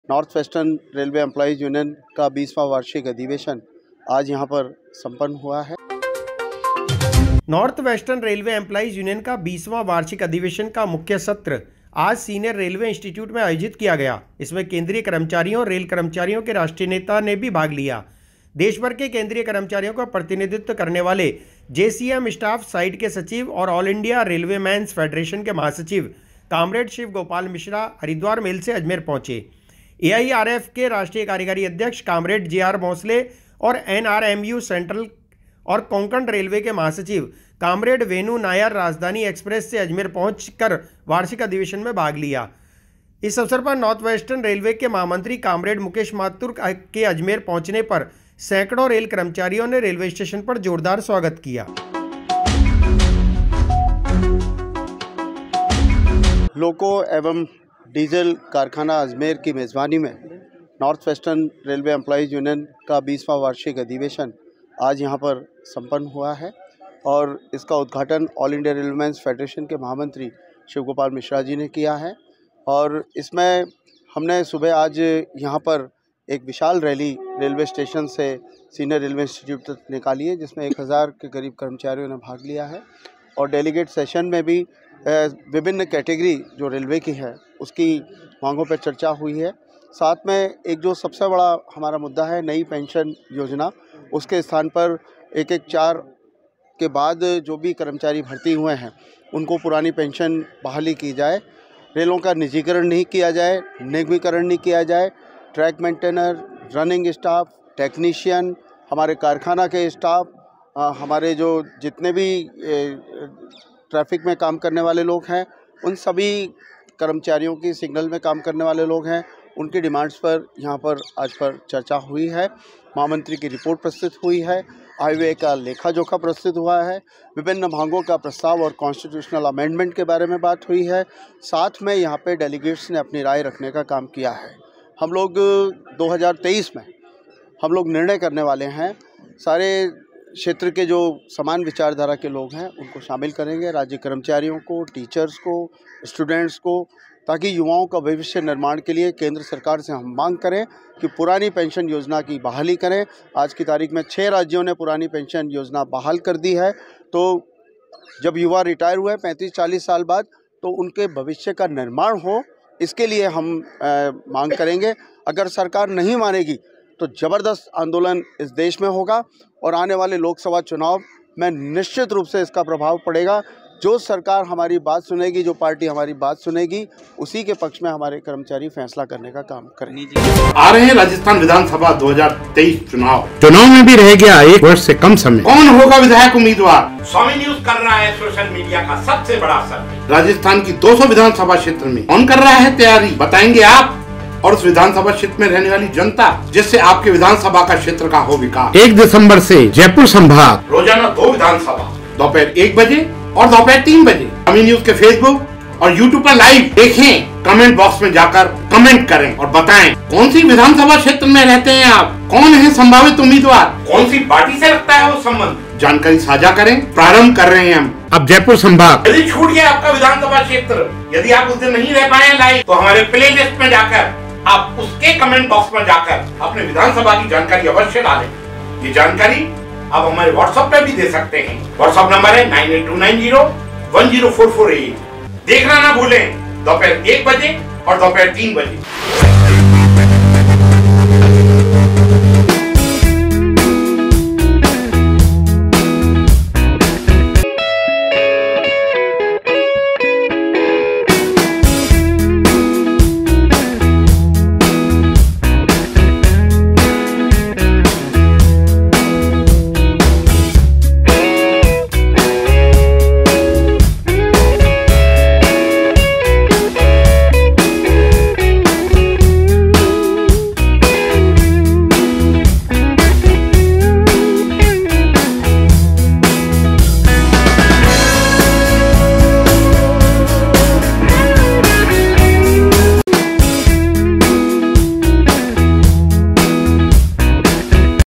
NWREU का 20 वां वार्षिक अधिवेशन
नॉर्थ वेस्टर्न रेलवे एम्पलॉईज यूनियन का 20 वाँ वार्षिक अधिवेशन आजनॉर्थ वेस्टर्न रेलवे एम्पलॉईज यूनियन का 20वाँ वार्षिक अधिवेशन का मुख्य सत्र, आज सीनियर रेलवे इन्स्टीट्यूट में आयोजित किया गया। इसमें केन्द्रीय कर्मचारियों एवं रेल कर्मचारियों के राष्ट्रीय नेता भी भाग लिया देश भर के केन्द्रीय कर्मचारियों का प्रतिनिधित्व करने वाले जे.सी.एम. स्टॉफ साईड के सचिव एवं ऑल इंडियां रेलवेमैन्स फैडरेशन के महासचिव कॉमरेड शिव गोपाल मिश्रा, हरिद्वार मेल से अजमेर पहुँच ए.आई.आर.एफ. के राष्ट्रीय कार्यकारी अध्यक्ष कॉमरेड जे.आर. भौसलें और एन.आर.एम.यू. सेन्ट्रल व कोंकण रेलवे के महासचिव कॉमरेड वेणु नायर, राजधानी एक्सप्रेस से अजमेर पहुँच वार्षिक अधिवेशन में भाग लेने के लिए नॉर्थ वेस्टर्न वेस्टर्न रेलवे के महामंत्री कॉमरेड मुकेश माथुर अजमेर पहुँचने पर सैकड़ों रेल कर्मचारियों ने रेलवे स्टेशन पर जोरदार स्वागत किया।